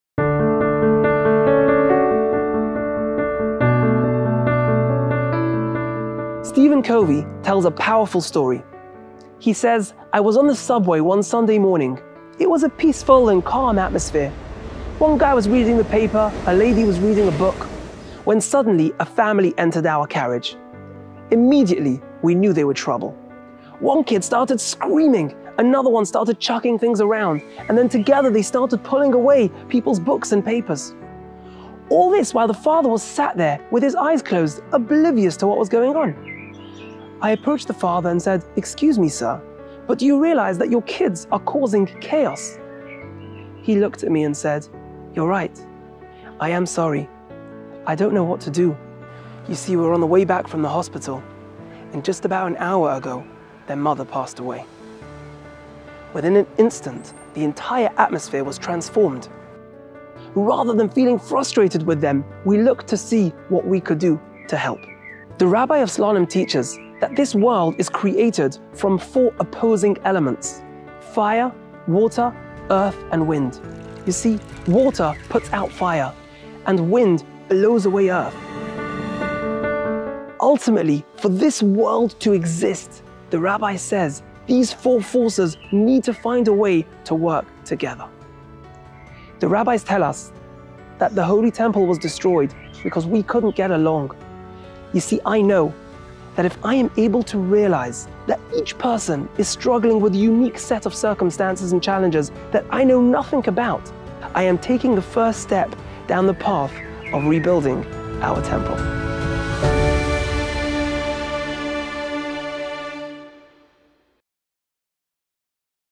Tammuz and Tolerance - Rabbi Aron Litwin
Tammuz and Tolerance - don't miss this extraordinary story re-told by Rabbi Aron Litwin and get inspired to be your best self this month...